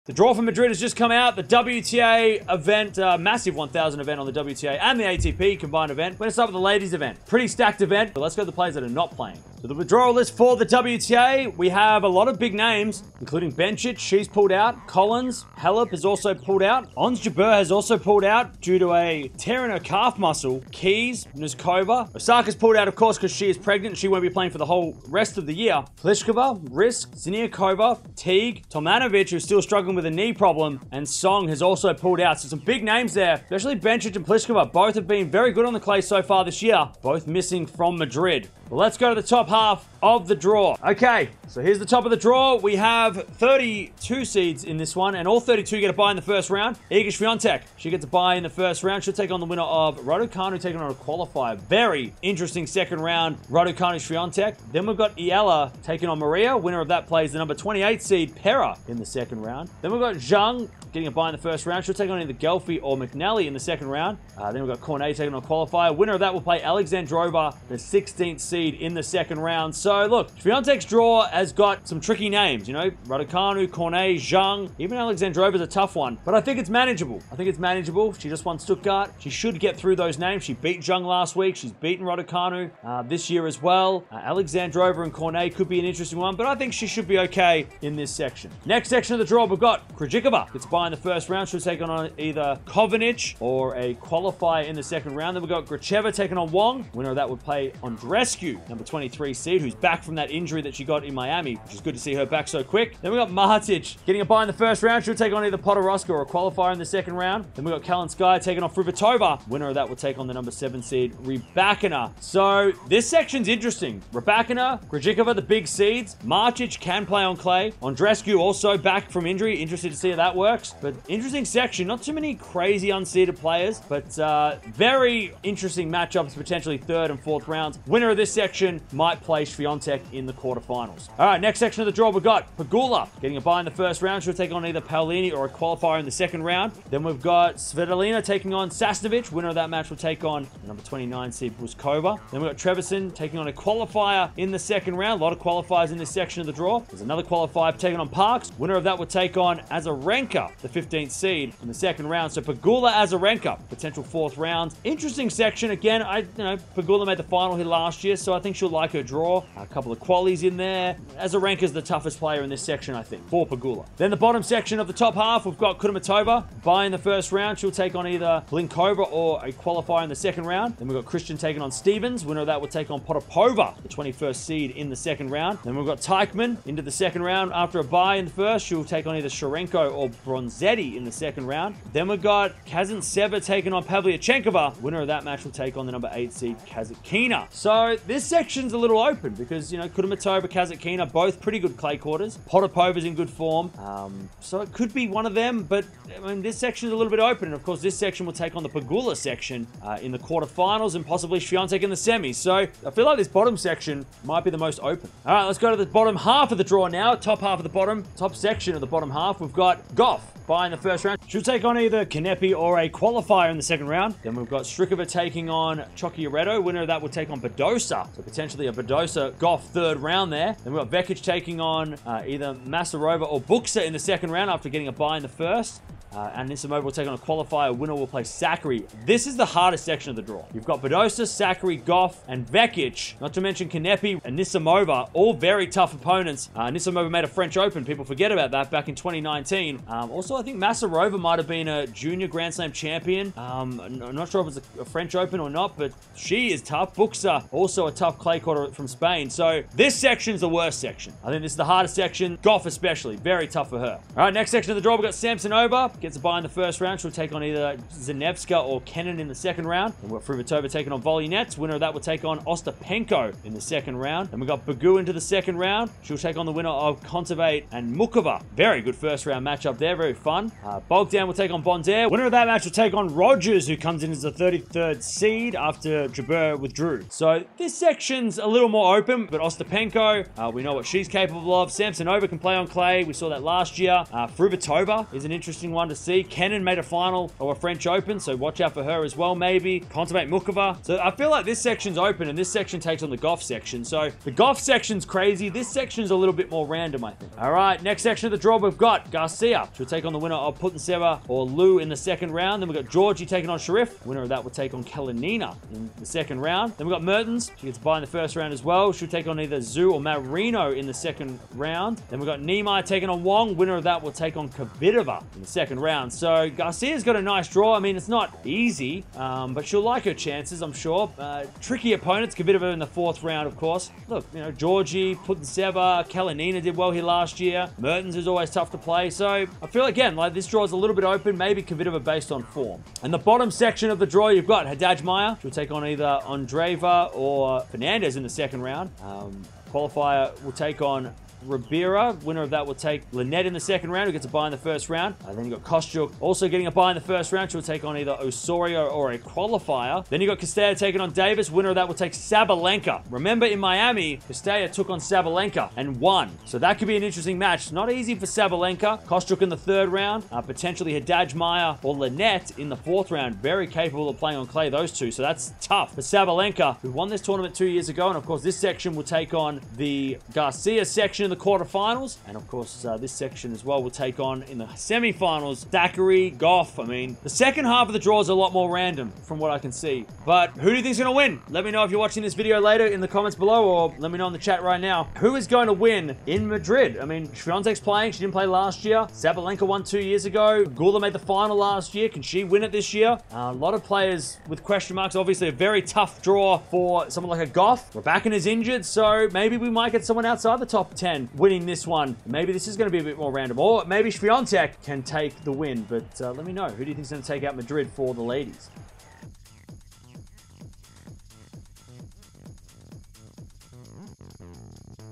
Swiatek Tough Draw at Madrid Open 2023 | Tennis Talk News
The Madrid Open 2023 official WTA draw preview. This event in Madrid, Spain will feature some of the best players in the world including Aryna Sabalenka, Barbora Krejickova, Emma Raducanu, Elena Rybakina and Iga Swiatek.

0:00 | Intro
0:09 | Withdrawals
0:42 | Section 1 | Swiatek #1
1:28 | Section 2 | Rybakina #7
2:11 | Section 3 | Pegula #3
2:44 | Section 4 | Kasatkina #8
3:25 | Section 5 | Gauff #6
4:14 | Section 6 | Ostapenko #19
4:57 | Section 7 | Garcia #5
5:33 | Section 8 | Sabalenka #2
6:20 | Predictions